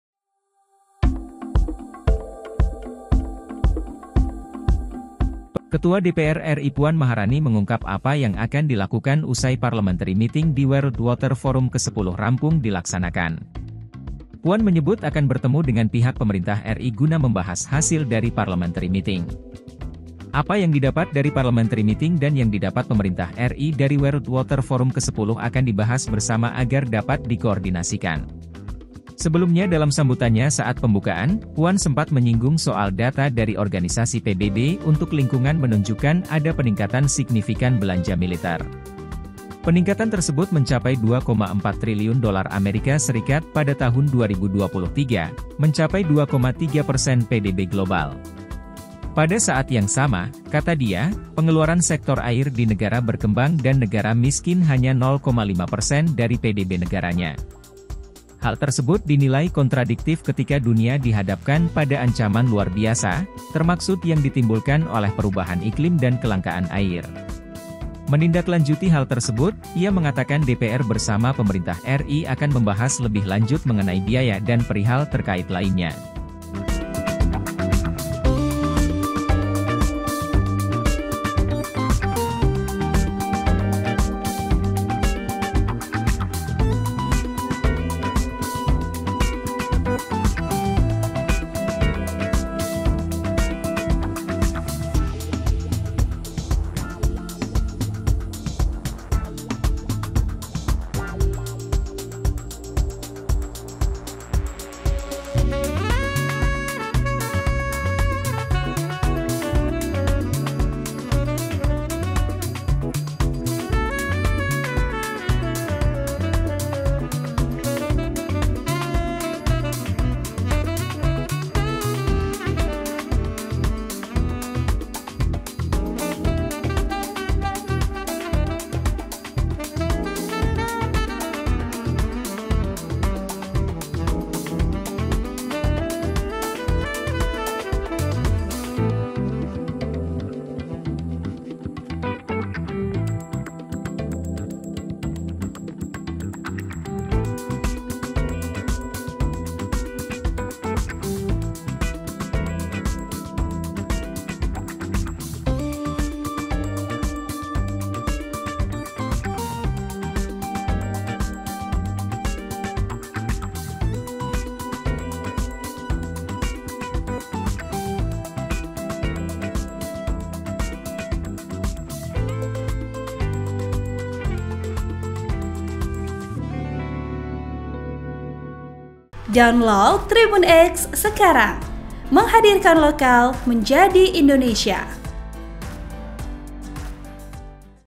DPR RI Akan Temui Pemerintah Untuk Bahas Langkah Berikutnya usai Parliamentary Meeting di WWF
TRIBUN-VIDEO.COM, BALI - Ketua DPR RI Puan Maharani mengungkap apa yang akan dilakukan usai Parliamentary Meeting di World Water Forum ke-10 rampung dilaksanakan.

Puan menyebut akan bertemu dengan pihak Pemerintah RI guna membahas hasil dari Parliamentary Meeting.

Apa yang didapat dari Parliamentary Meeting dan yang didapat Pemerintah RI dari World Water Forum ke-10 akan dibahas bersama agar dapat dikoordinasikan.

SELENGKAPNYA
Artikel ini telah tayang di Tribunnews.com dengan judul Usai World Water Forum Ke-10, DPR Bakal Temui Pemerintah Untuk Menentukan Langkah Berikutnya,
Penulis: Endrapta Ibrahim Pramudhiaz
Editor: Hendra Gunawan
Video Production: TSM